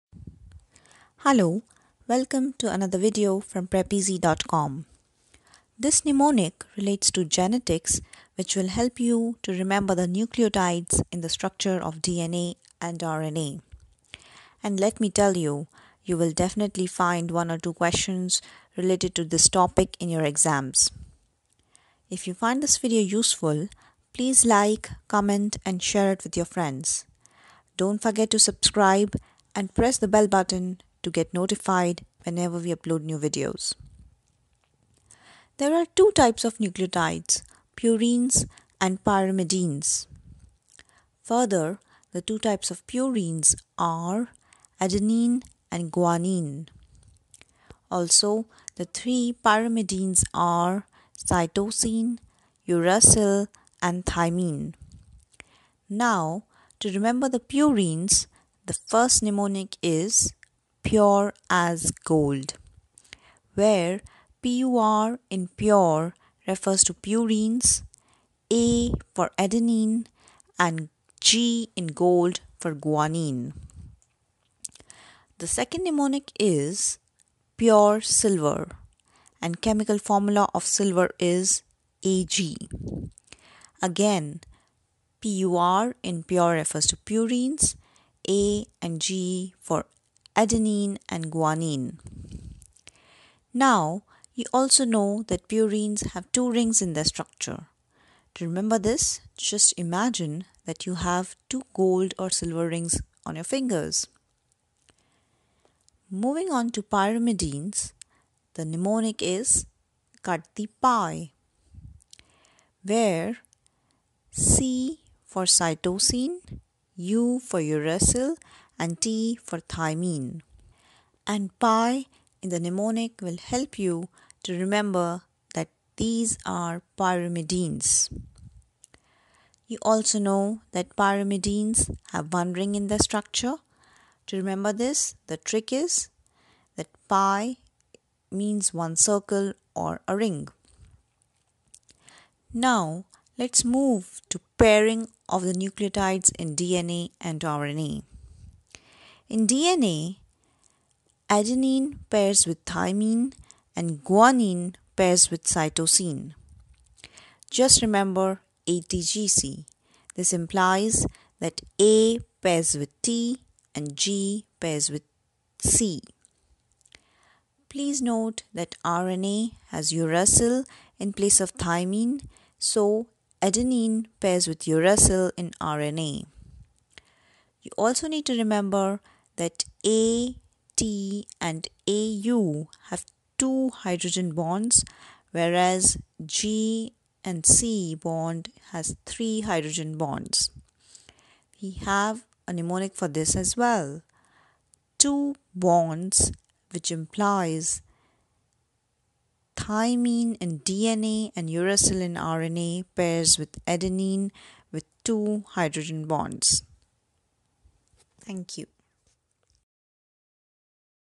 Memorize Nucleotide in Structure of DNA & RNA in 2mins
Learn Nucleotide in the structure of DNA and RNA using mnemonics.
There are two types of nucleotide- Purines and Pyrimidines.
The video also helps you to remember base pairing of nucleotide.

We really need your appreciation and support to continue making similar videos. Also, feel free to request mnemonics for the topics you are struggling with.